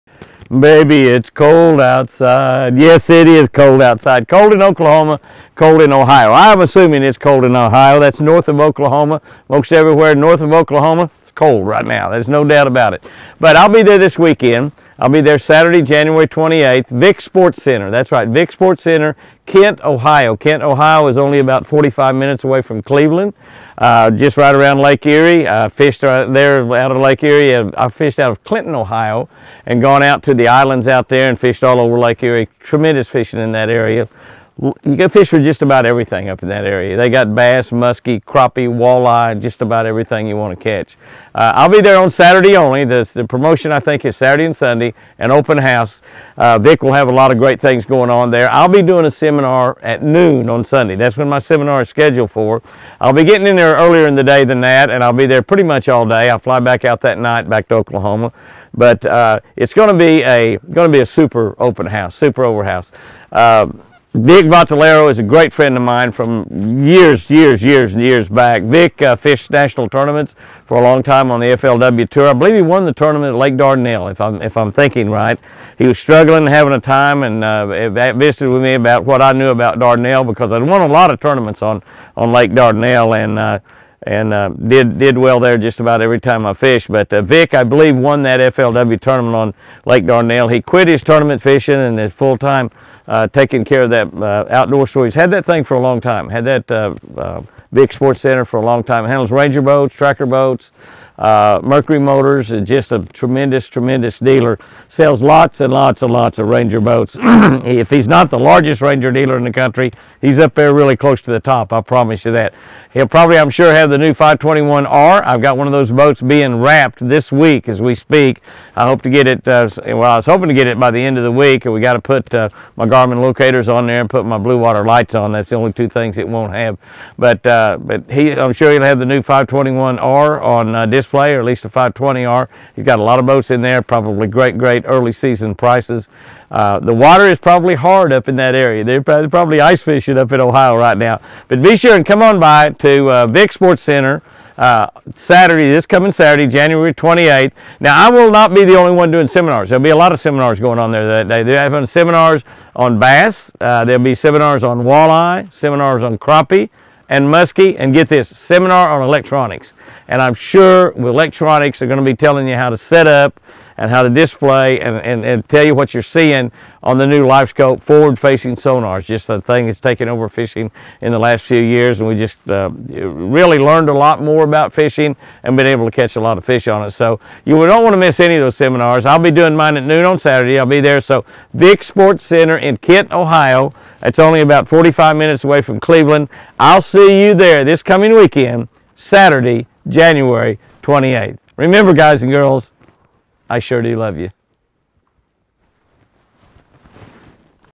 I’m in Ohio This Saturday!!! Come Visit!!
Vic's Sports Center, Kent, Ohio!   Seminar at Noon!!!   Don Miss This One!!!